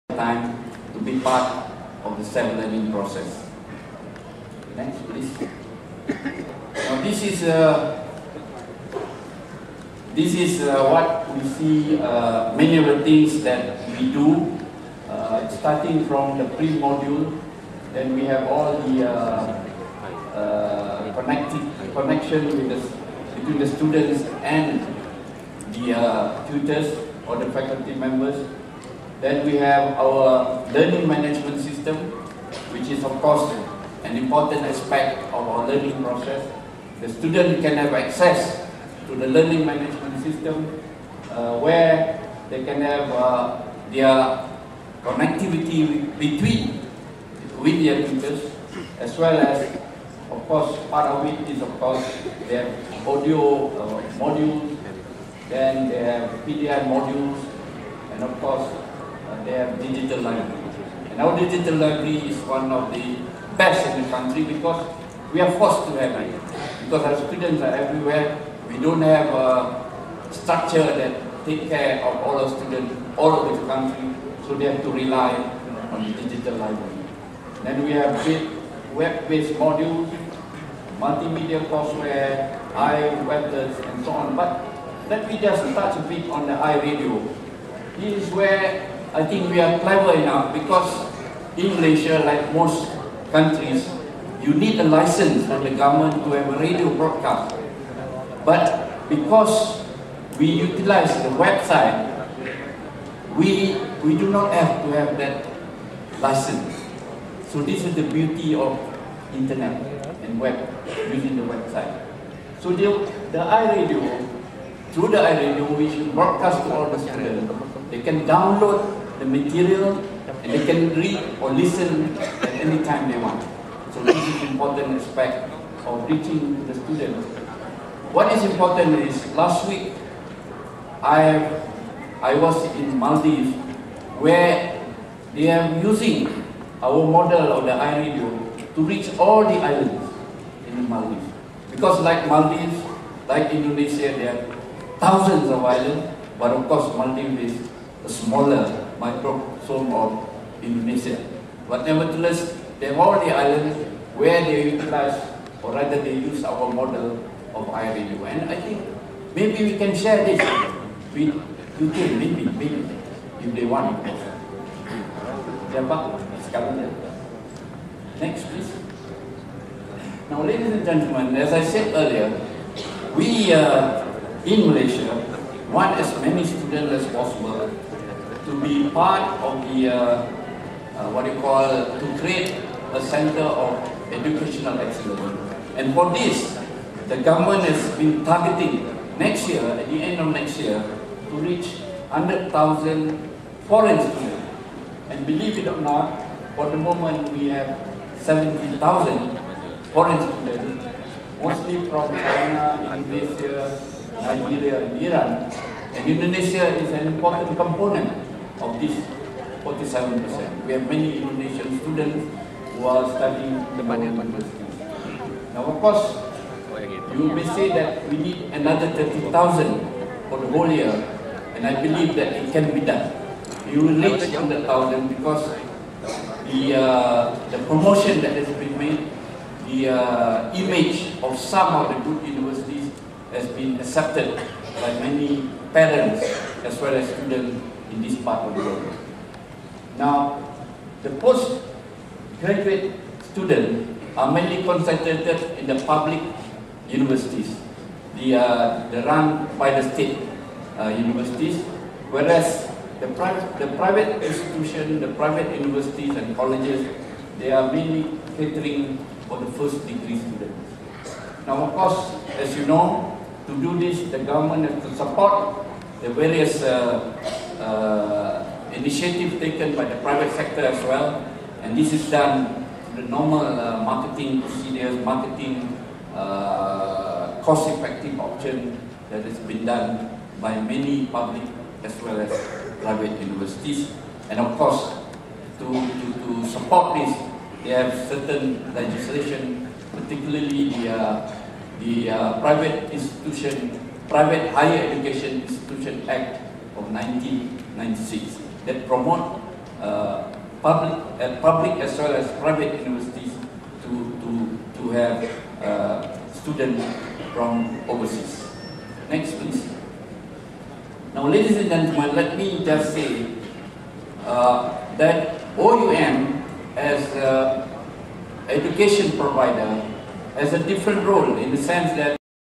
Tan Sri Anuwar - Part 3.flv
Presentation by Tan Sri Anuwar Ali as plenary speaker ISODEL 2009 - video by Richard Ng Dec 14 2009